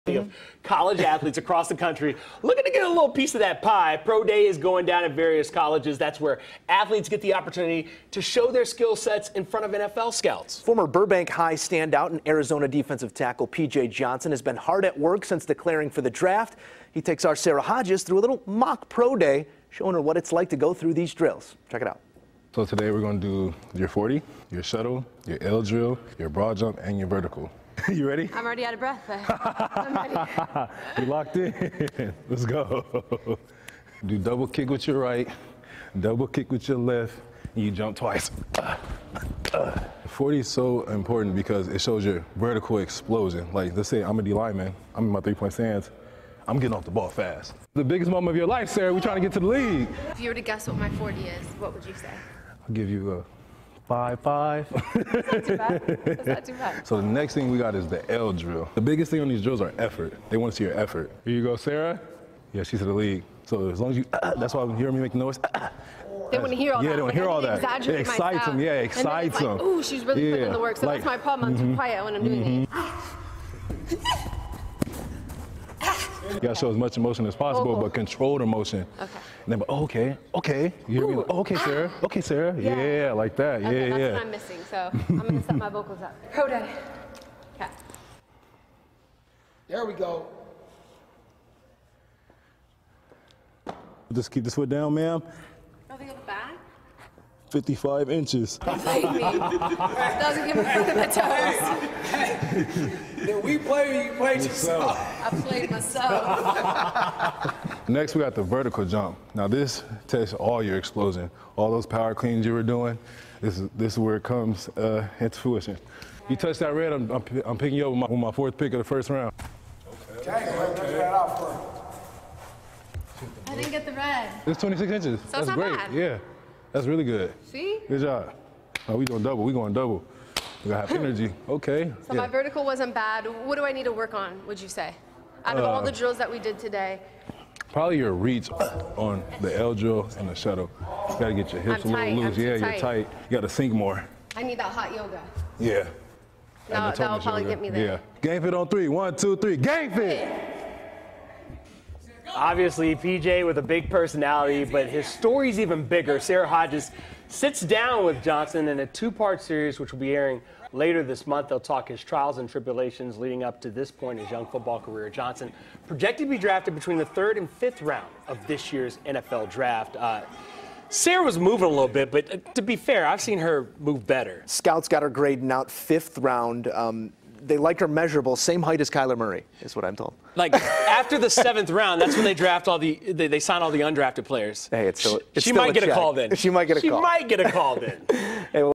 Scouting Combine
Sara Hodges gets ready for the NFL scouting combine.